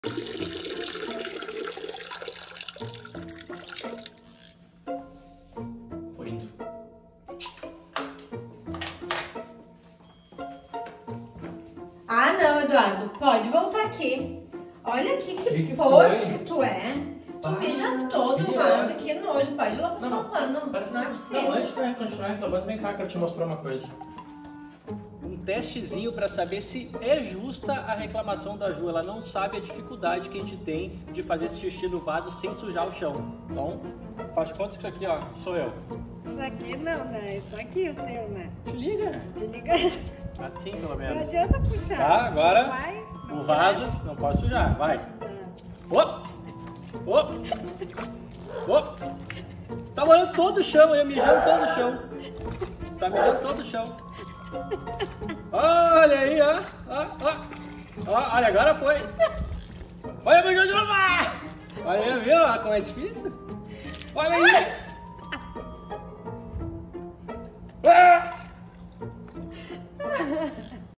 EXPLICANDO A JU COMO É DIFÍCIL FAZER XIXI
Acho que agora ela entendeu exatamente as dificuldades que temos